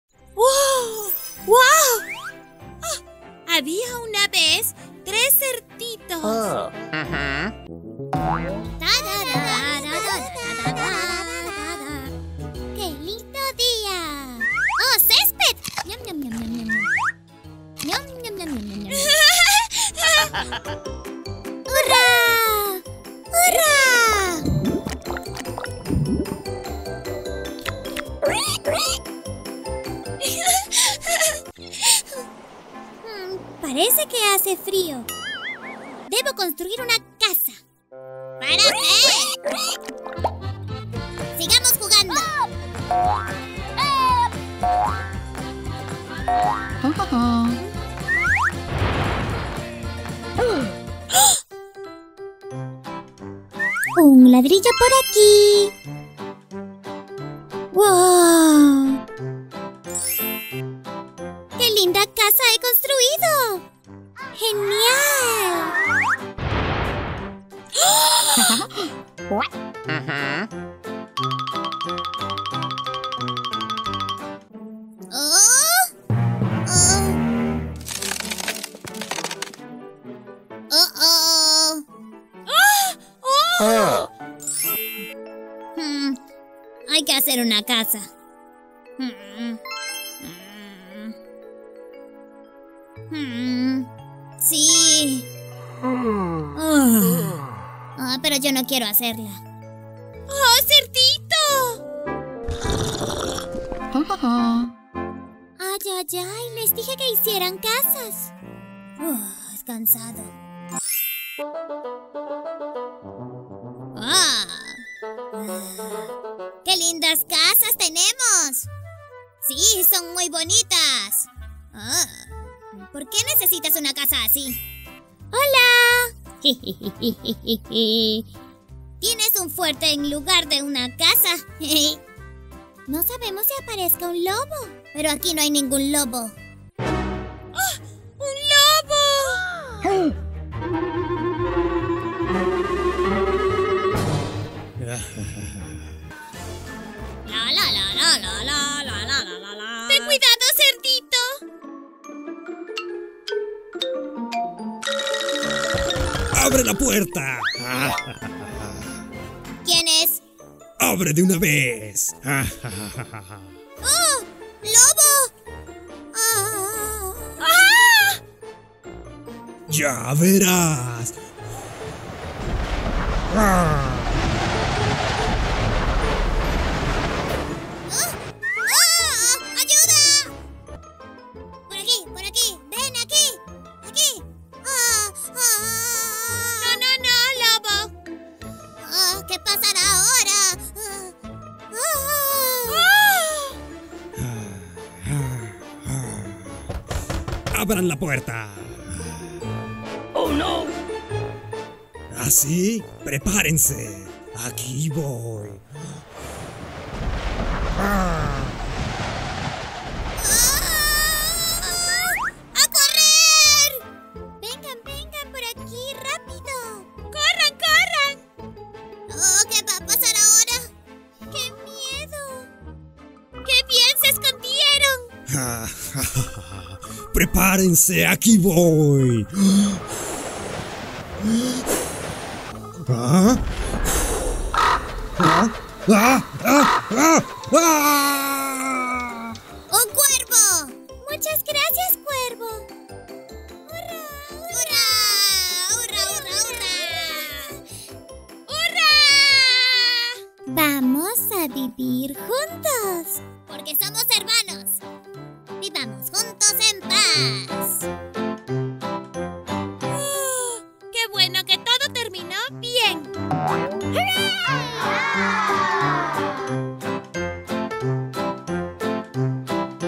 Alicia Lee cuentos graciosos sobre Tres Cerditos
Es una historia graciosa como Alicia le Lee cuentos a sus juguetes. Tres cerditos están construyendo su casa.